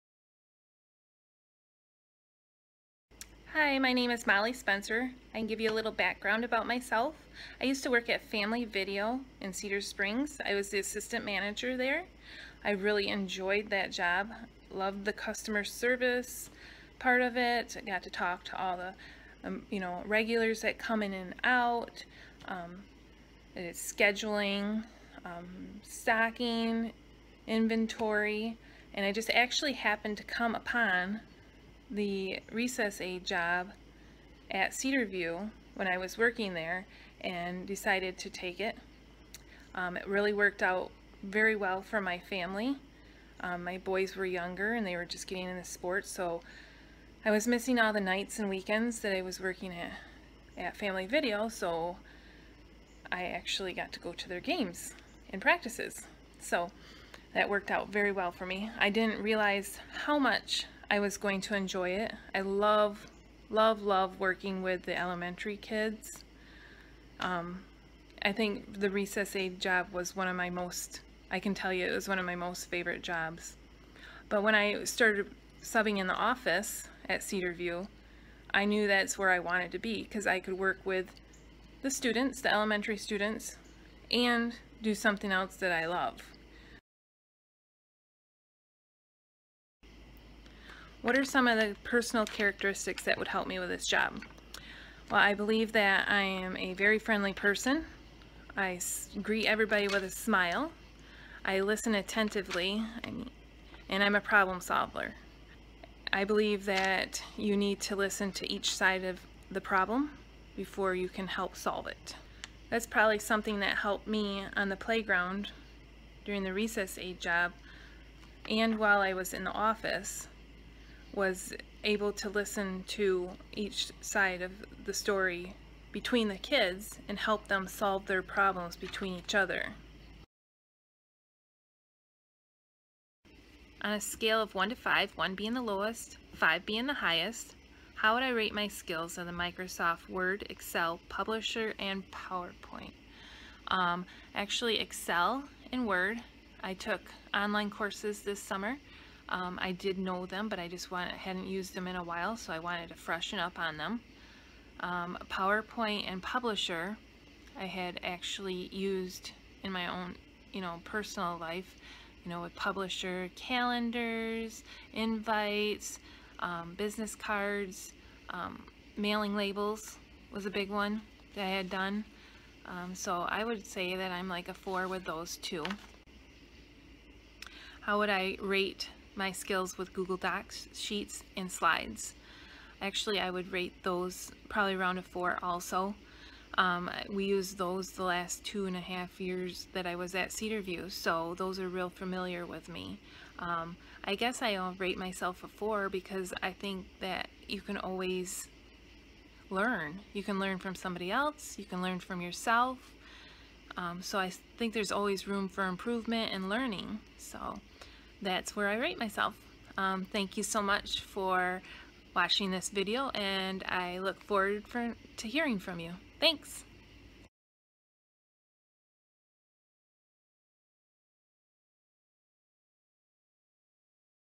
Video Interview
My video for the secretarial position at Cedar Springs Public Schools.